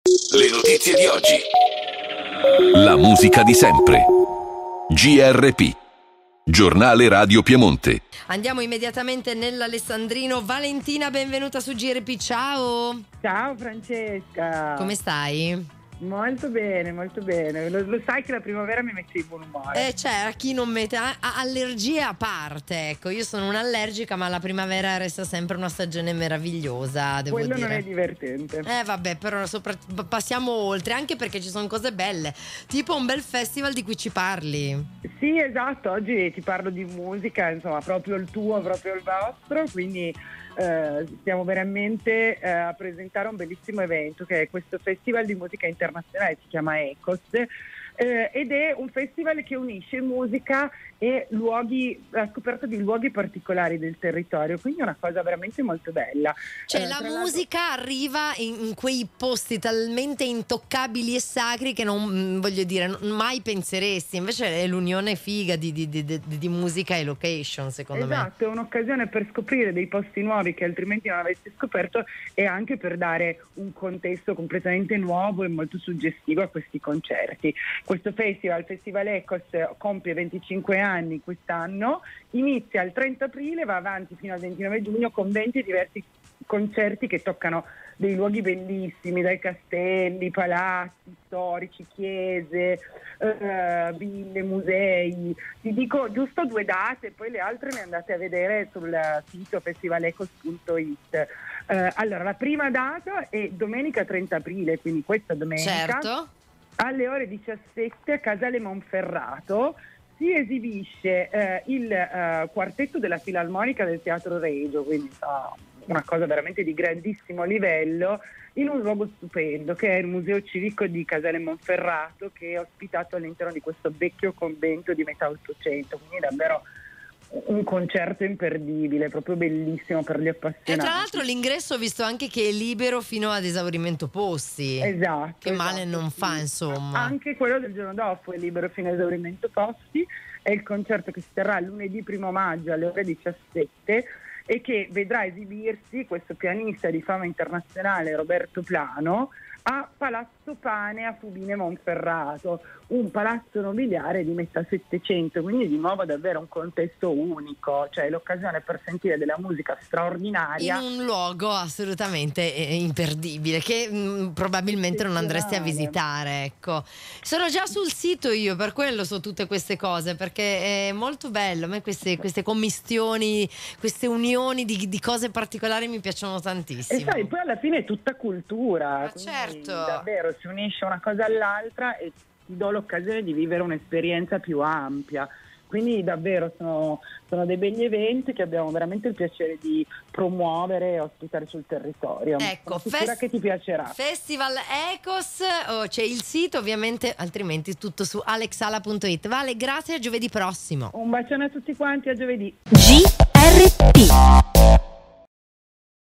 FESTIVAL ECHOS
Il Festival “Echos. I Luoghi e la Musica”, giunto alla XXV edizione, è organizzato dall’Associazione Musicale Ondasonora APS di Alessandria.
Ondasonora attraverso il Festival Echos si propone di valorizzare il ricco patrimonio storico, artistico e paesaggistico della provincia di Alessandria ambientando concerti di altissima qualità in castelli, chiese, palazzi e teatri storici, pievi, abbazie, musei e altro.